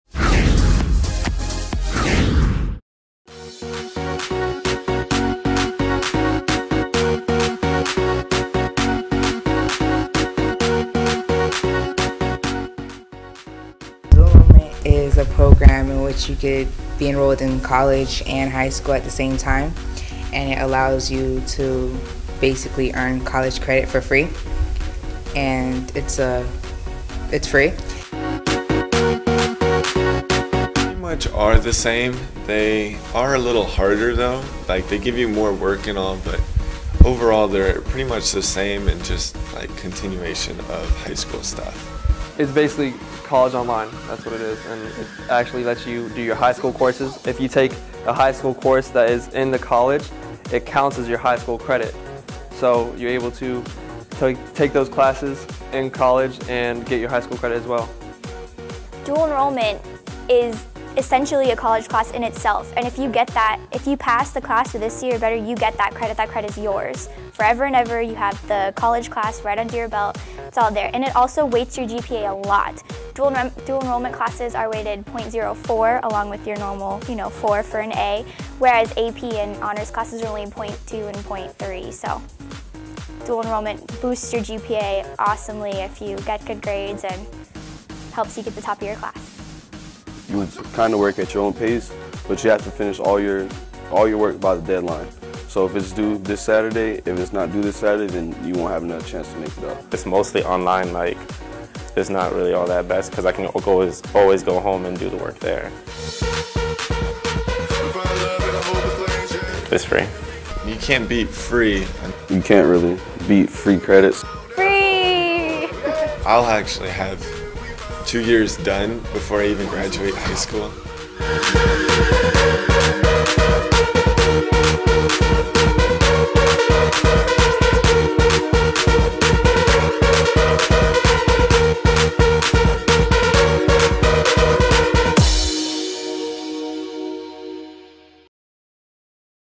Dual Enrollment at Lehigh Senior High School.avi
Dual Enrollment at Lehigh Senior gives students the opportunity to earn college credit for FREE while they are taking high school classes. With dedication and hard work, students at Lehigh can earn enough college credits to graduate with their Associate Degree at no cost, and be well on their way to earning their four-year Bachelor Degree, before they graduate high school.